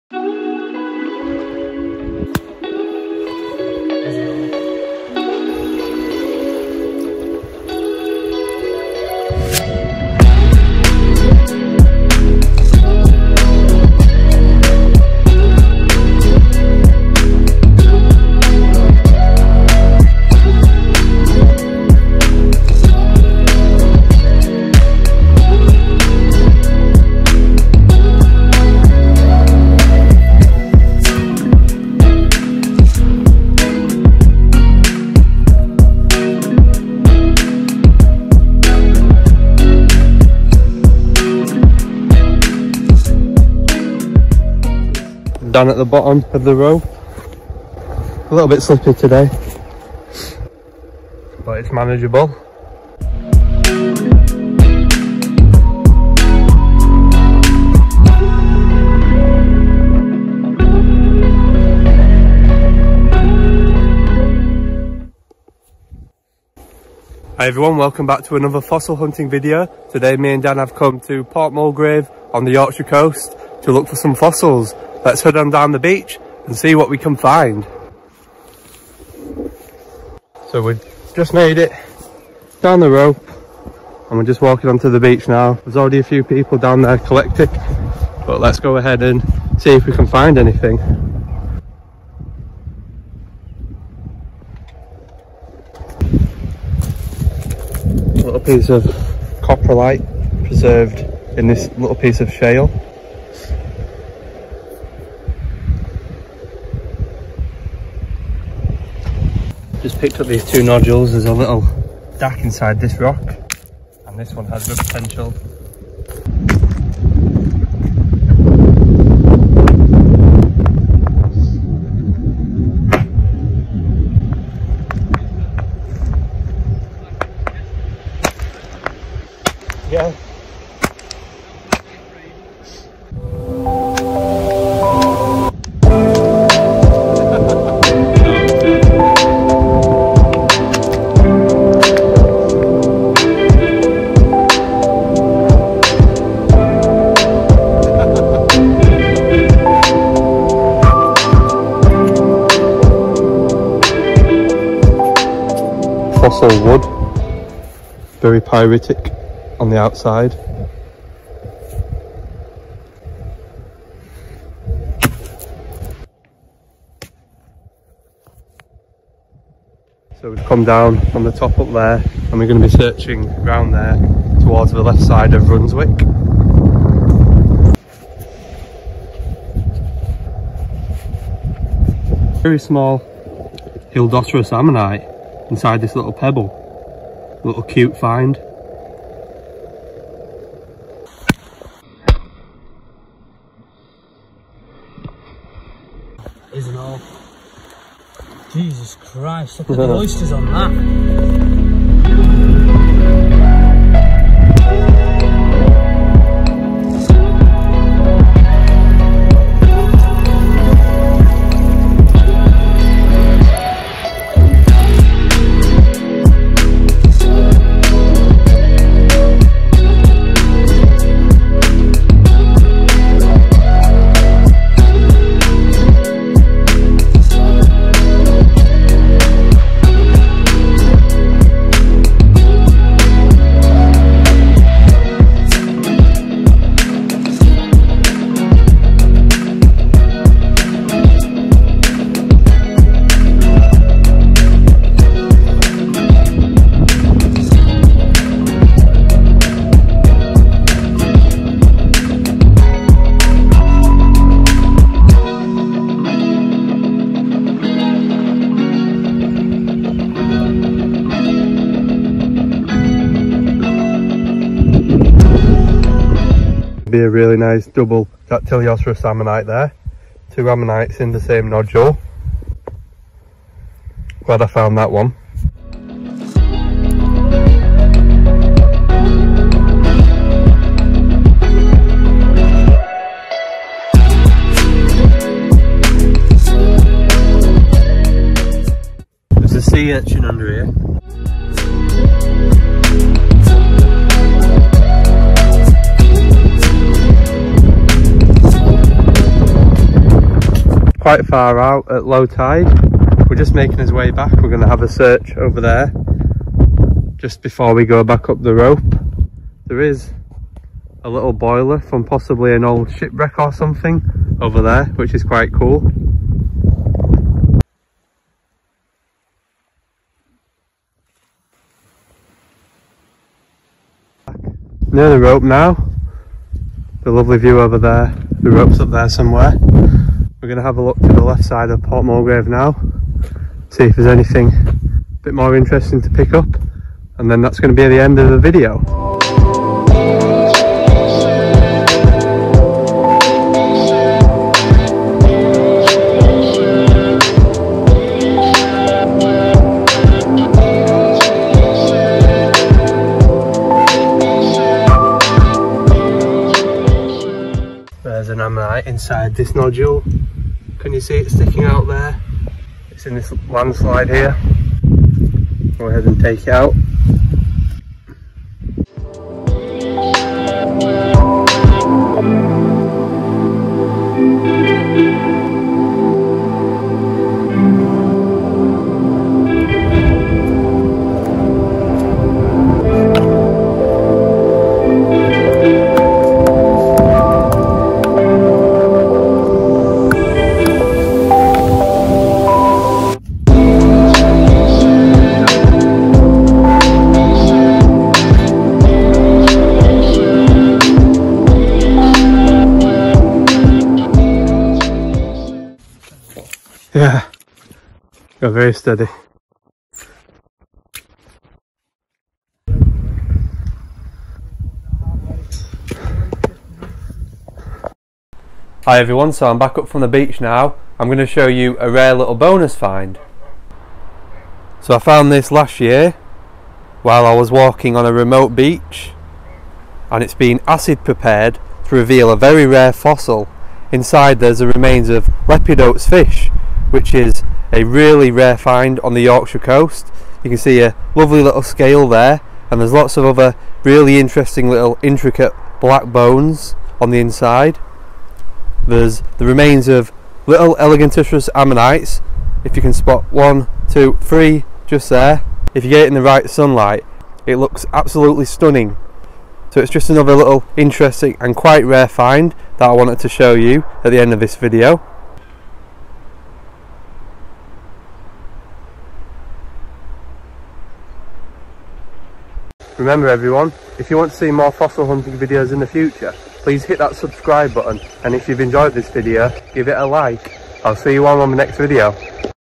Fossil Hunting At An Abandoned Ironstone Port
Today I go fossil hunting at an abandoned ironstone port. Port mulgrave at Yorkshire. We look for ammonite fossils and later I show an amazing and rare fossil fish find I found earlier in the year

✅ Recommends